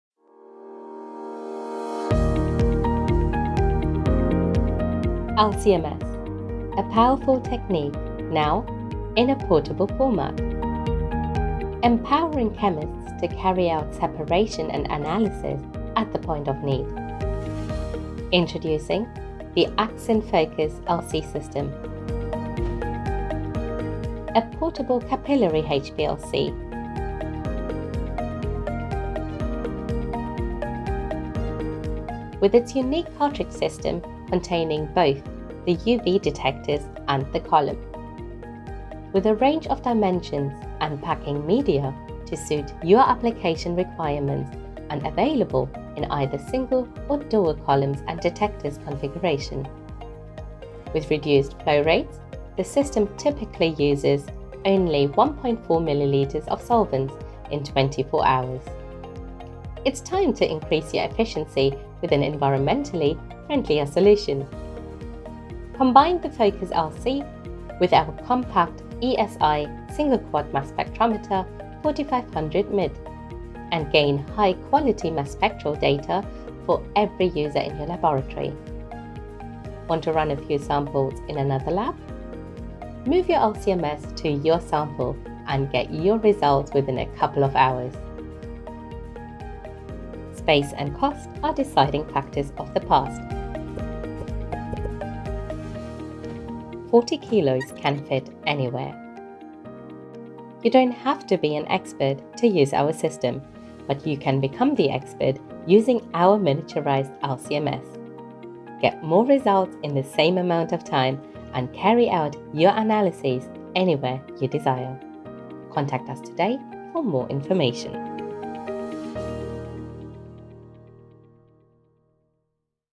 Microsaic Systems introduces the Axcend Focus LC®
Microsaic Systems is delighted to welcome the Axcend Focus LC® to our product range. The global collaboration of the Microsaic 4500 MiD® and the Axcend Focus LC® enables chemists to carry out both separation and analysis at the point of need.

The small footprint technologies support timely testing in the location where samples are taken, as well as in centralized laboratories.  In addition, the combined LC/MS system utilizes substantially fewer solvents than the traditional LC and MS systems, which can, therefore, result in lower running costs and solvent disposal.